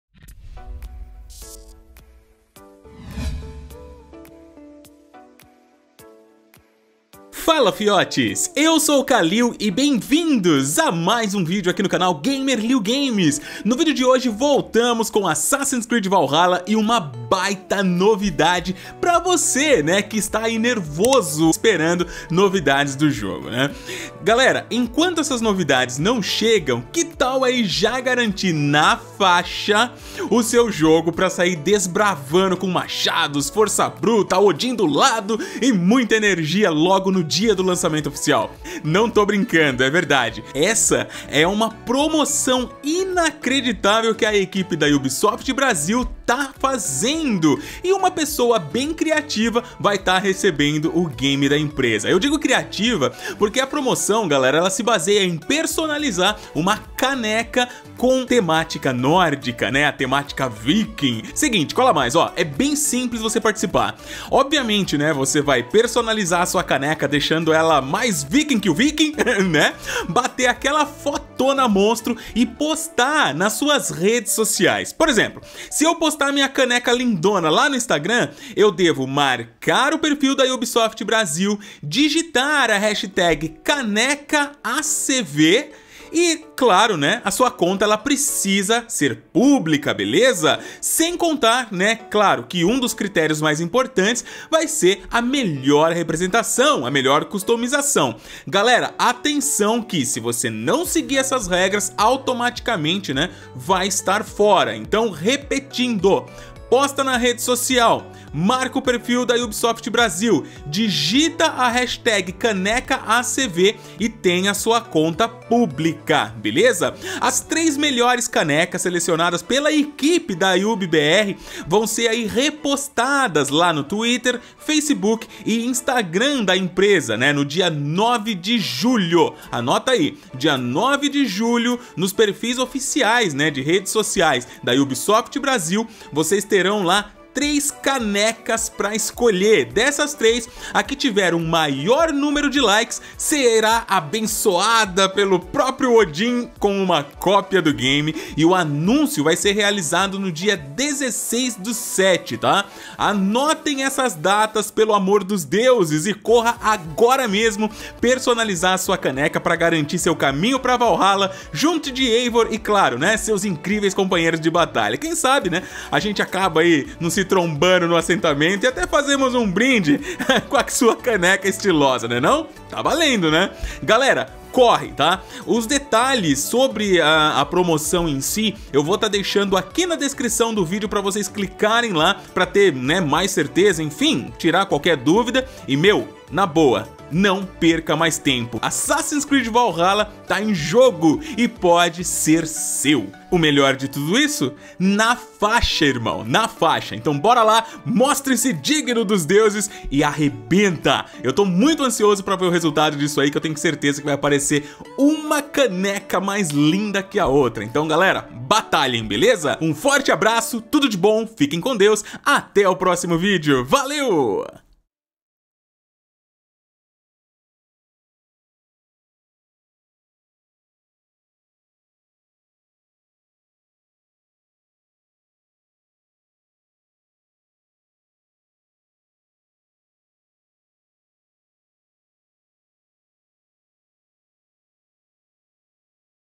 Assassin's Creed Valhalla - Ganhe o Jogo na Faixa!!!! Promoção Brindando como Viking
Fala Fiotes!
Como estão?
Promoção Imperdível da Ubisoft Brasil vai te dar uma copia de Assassin's Creed Valhalla no dia do seu lançamento e para participar é muito fácil e divertido! Venha Brindar como um Viking!

PARCEIROS
Loca Jogos

Kallil Augusto